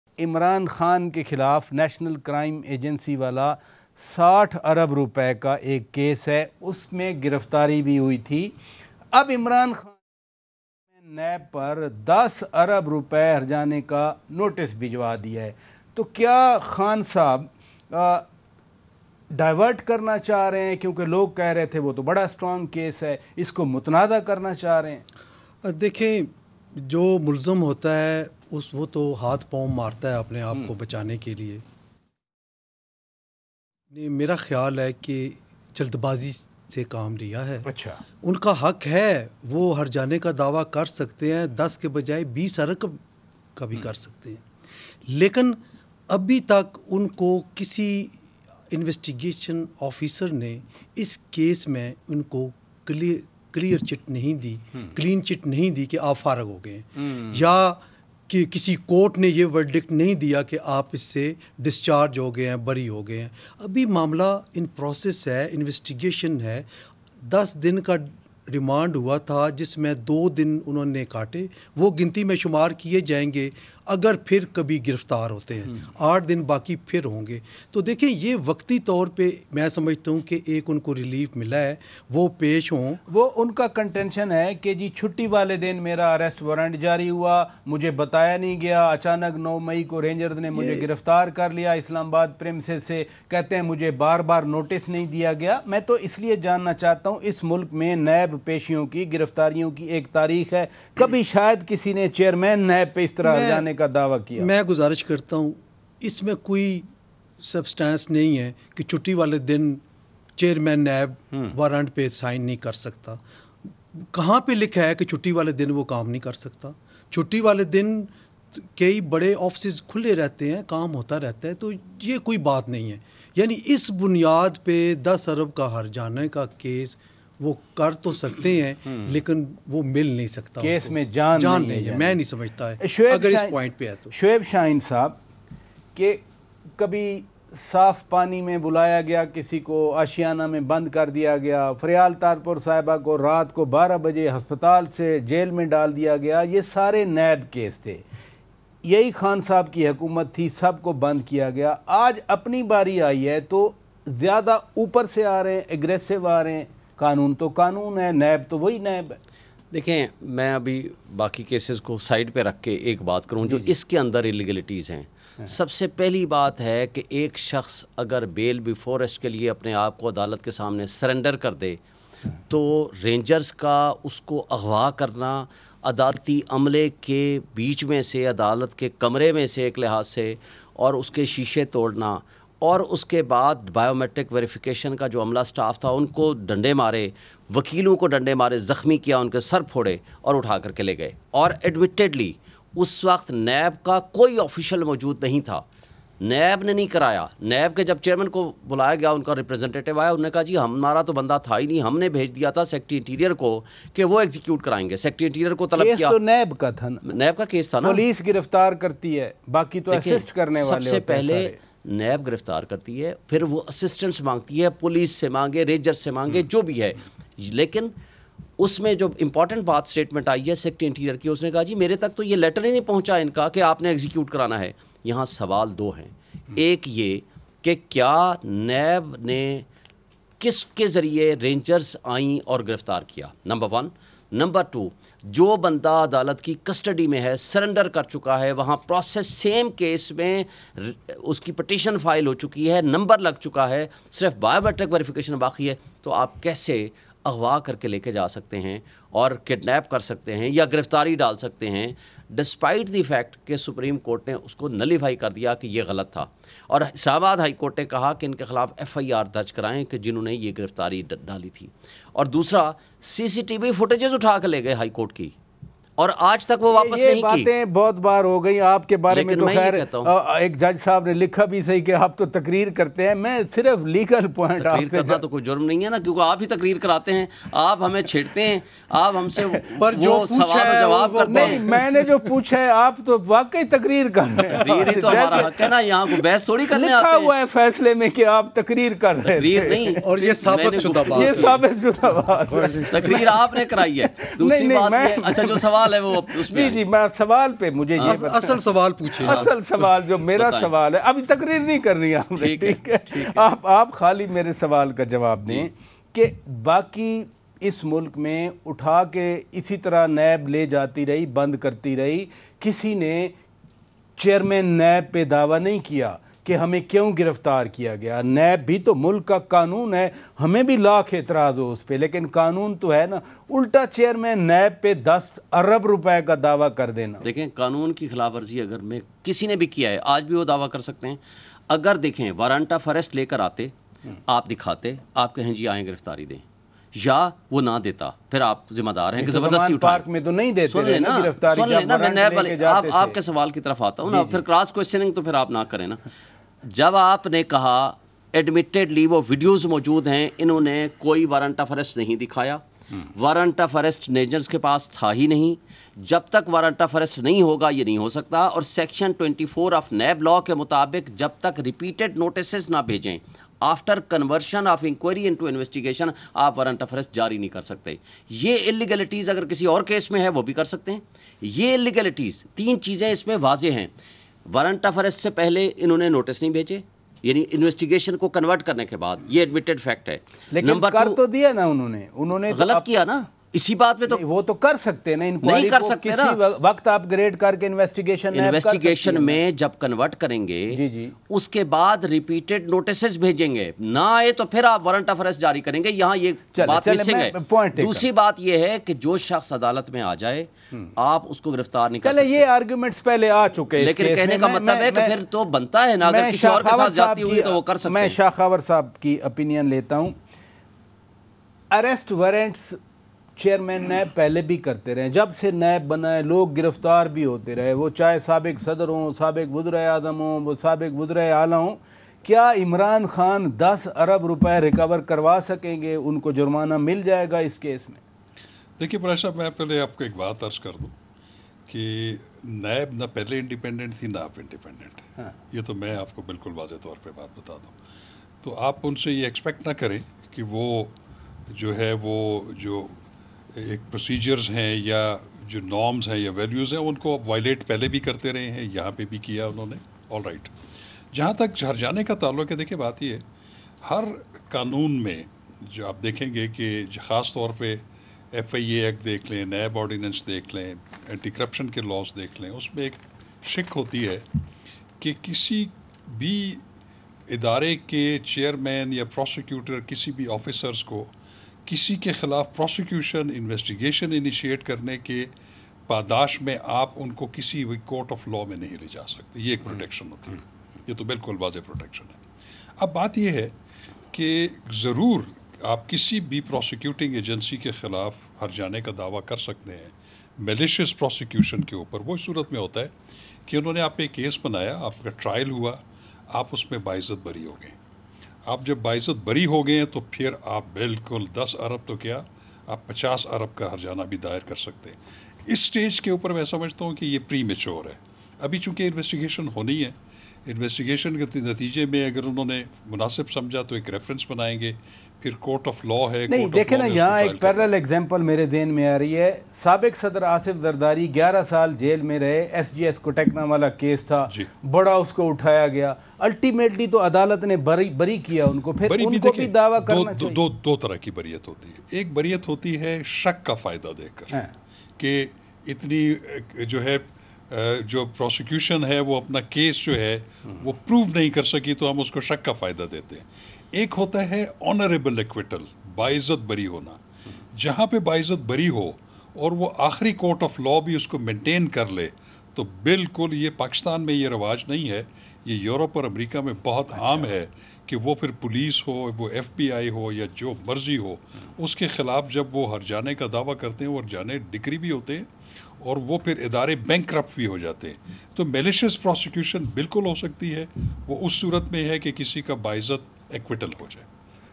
Is it premature for Imran Khan to claim damages on NAB?| Aaj News
Aaj TV Official:
shorturl.at/iBDNX